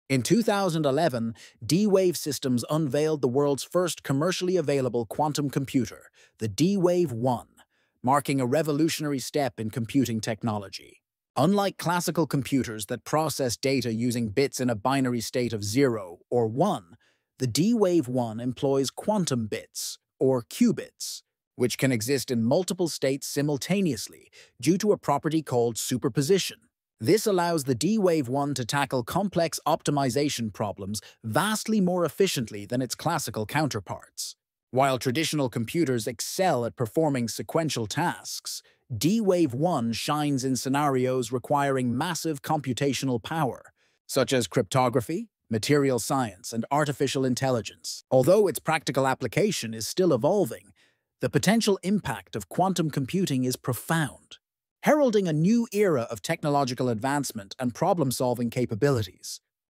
Quantum Leap: D-Wave One Transforming Computing Forever!
Unlock a glimpse into the future with D-Wave Systems' groundbreaking innovation, the D-Wave One, introduced in 2011 as the world's first commercially available quantum computer. Unlike traditional computers that utilize a binary system of zeros and ones, the D-Wave One operates with qubits, leveraging the quantum phenomenon of superposition. This unique capability allows the D-Wave One to handle complex optimization problems with unparalleled efficiency. While classical computers are adept at sequential processing, the D-Wave One excels in fields demanding extensive computational power, such as cryptography, material science, and artificial intelligence. As we stand on the brink of a new technological era, the transformative potential of quantum computing continues to evolve, promising profound advancements in problem-solving capabilities.